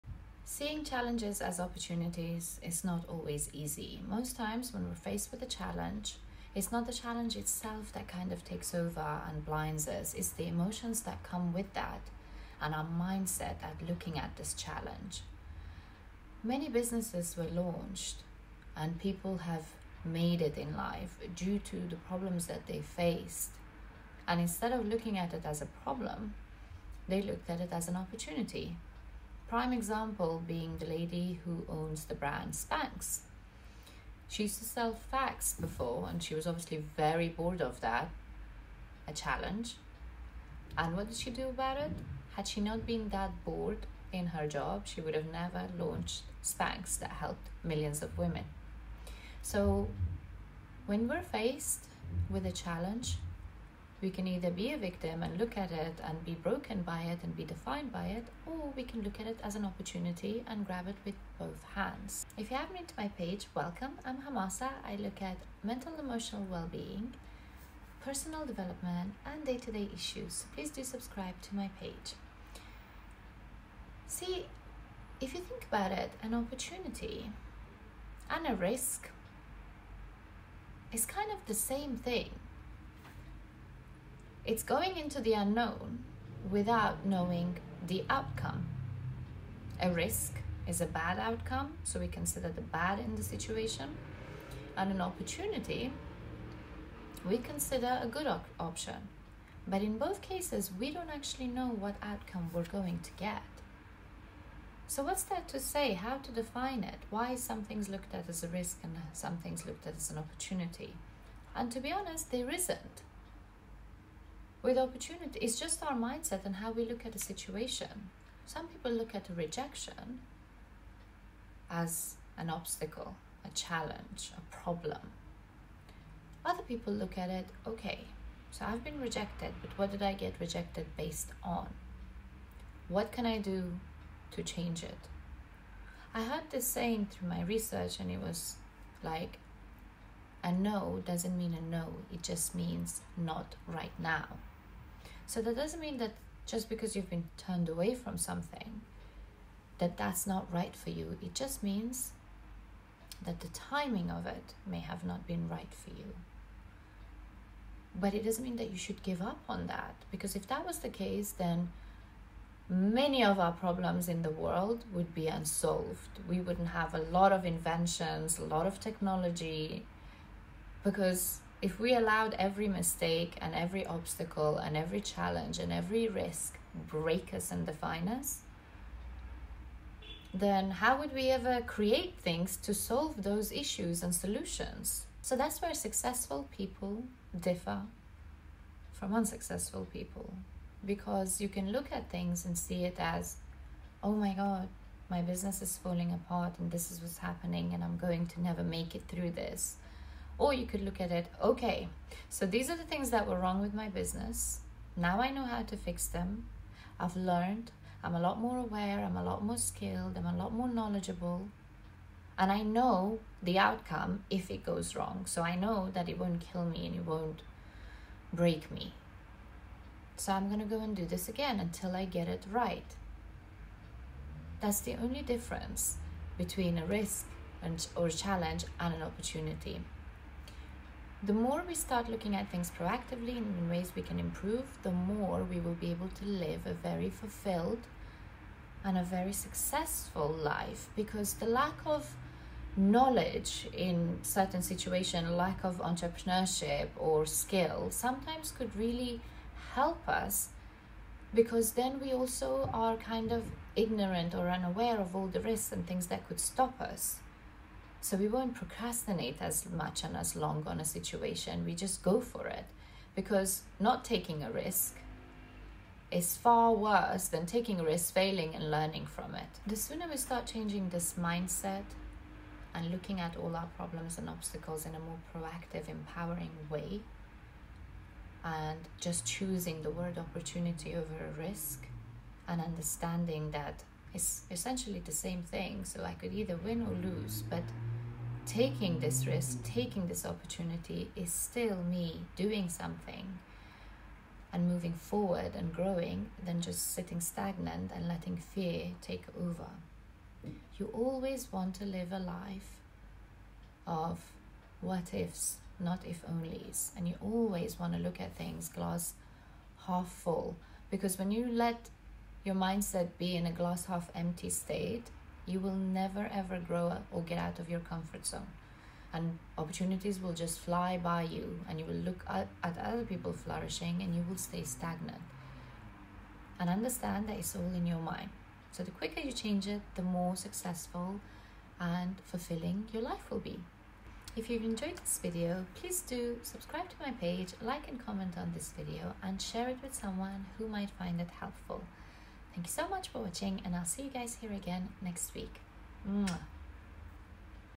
Seeing Challenges as Opportunities
When we're faced with challenges we often feel broken and give up.
But what if we look at thing differently and use it as an opportunity to make something out of it.